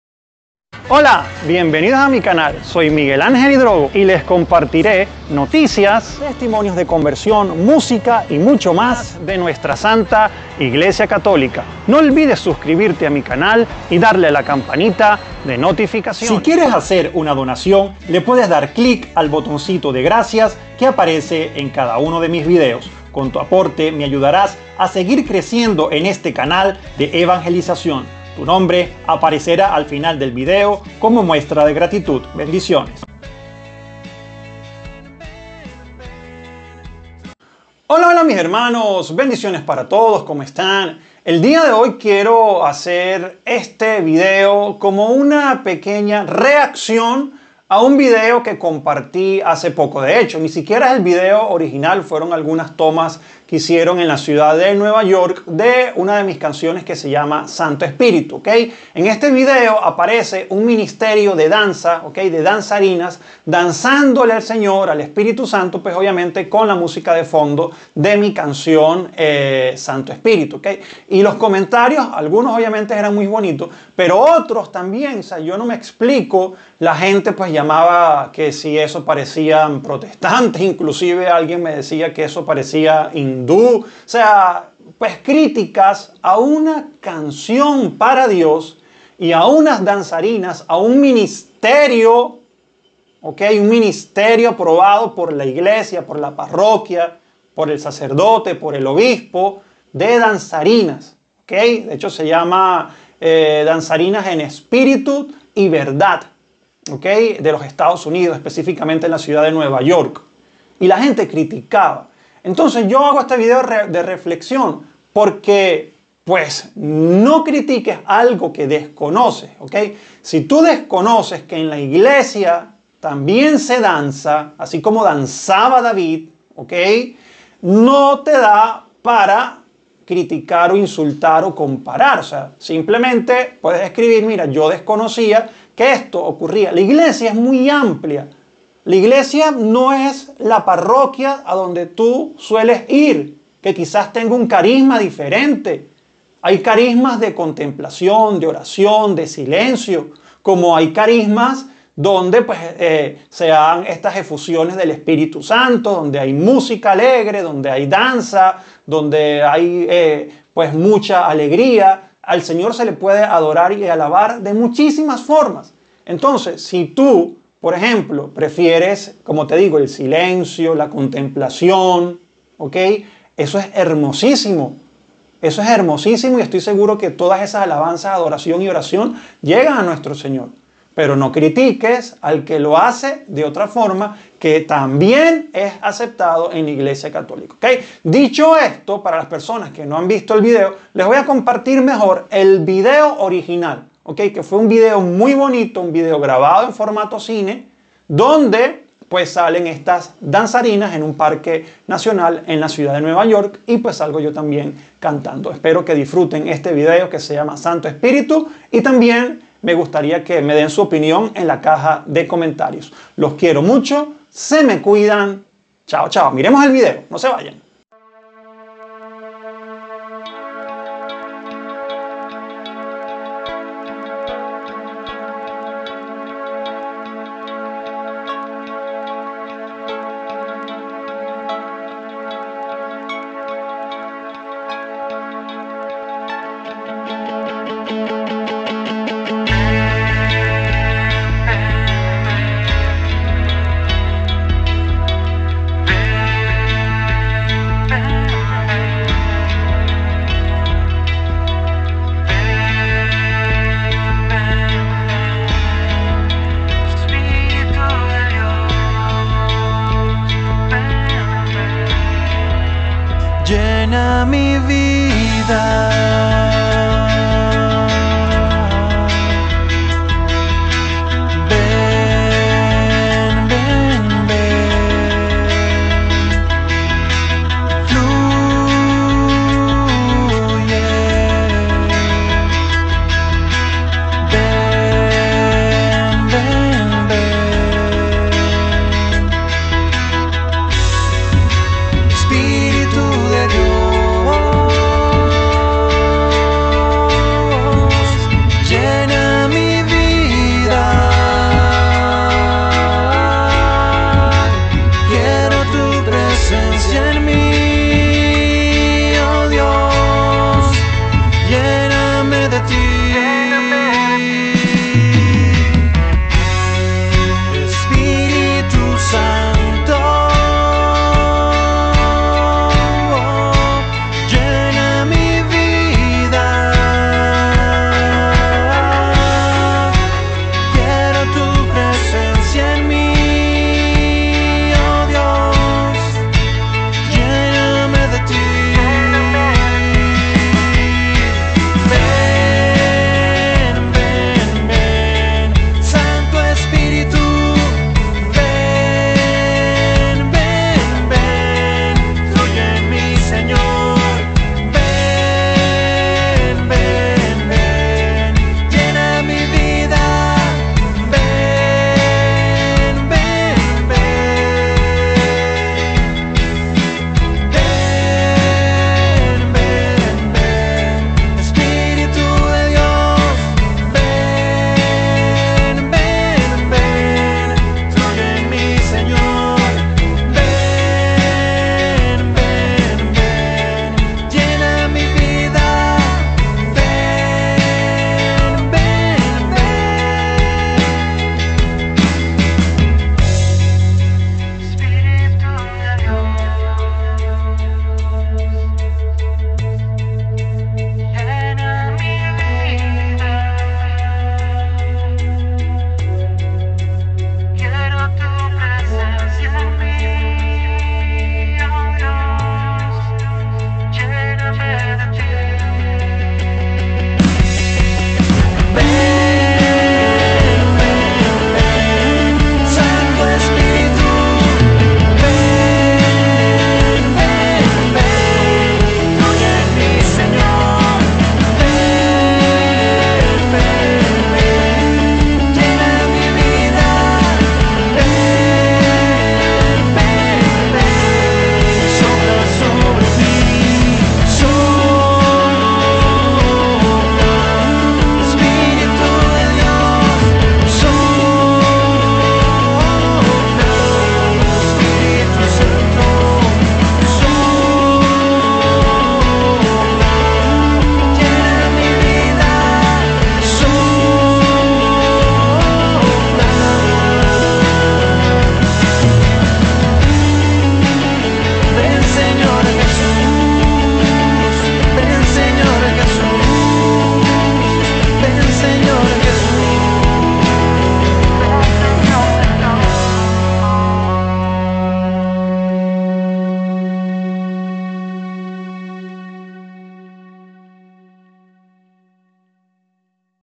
¿Me voy de la IGLESIA? ¿Me hice Protestante? me Critican por un vídeo ¿Pero de que se trata? 😱🤯
Me han criticado por un vídeo musical donde sale un ministerio de danza CATÓLICO, Adorando al señor.